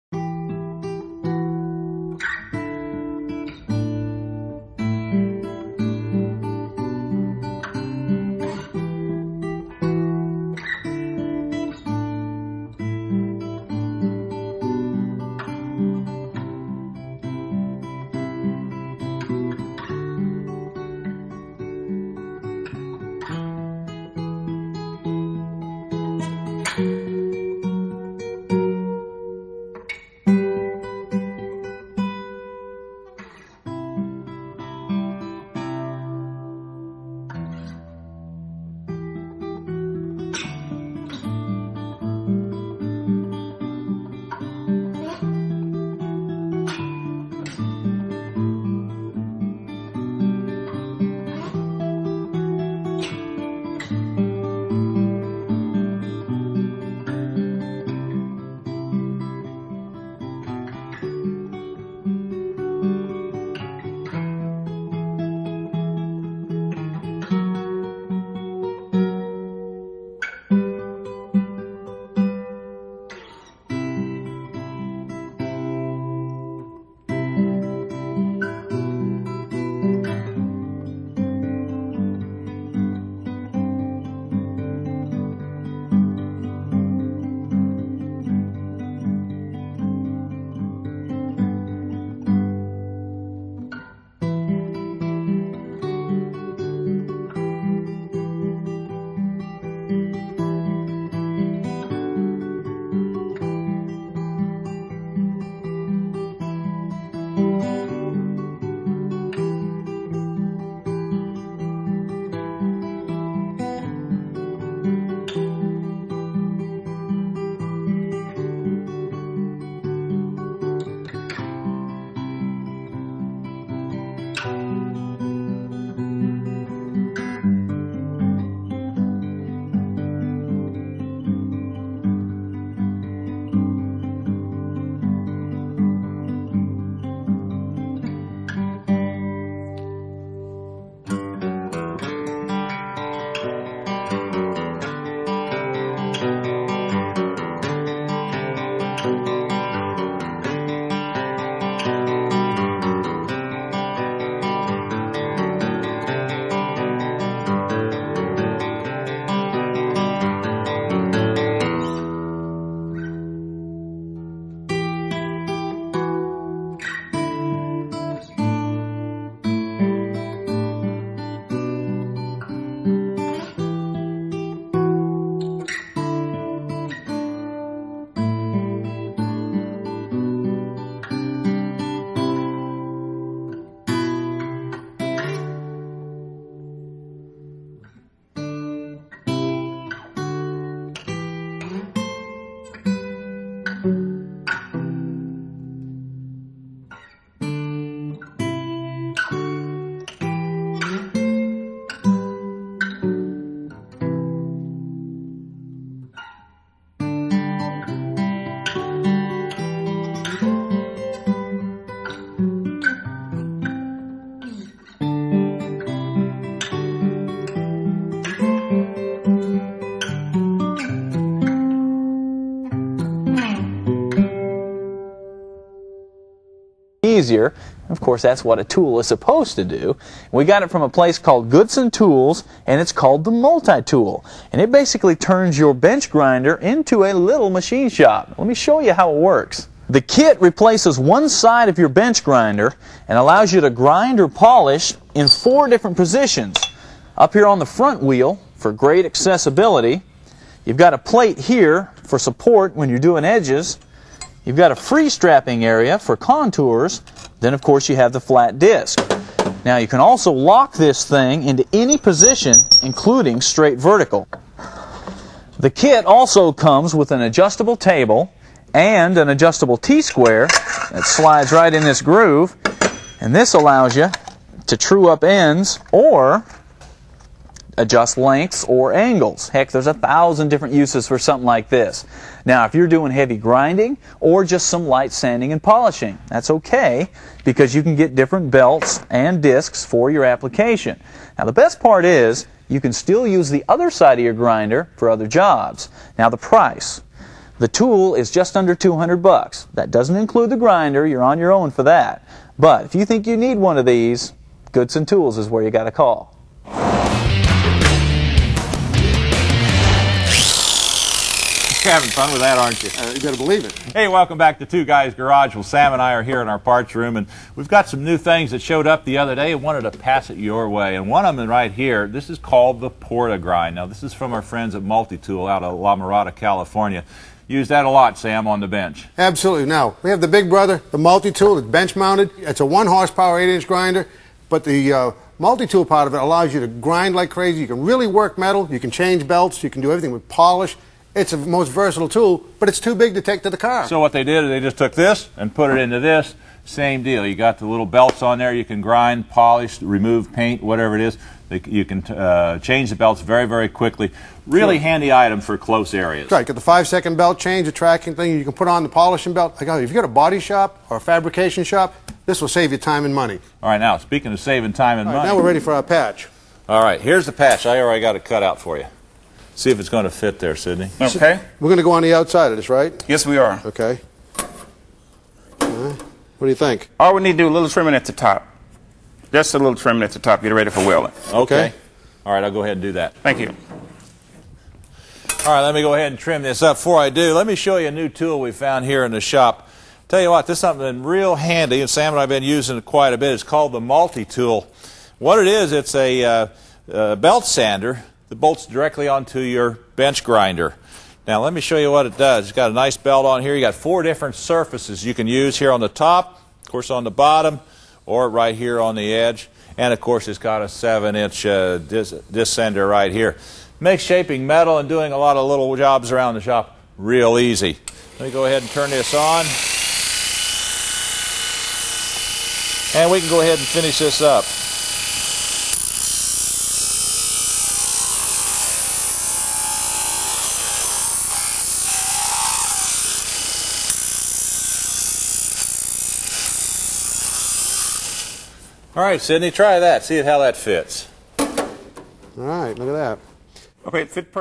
Multitool TV Shows
Here are a series of clips from some of the TV shows the Multitool was shown on.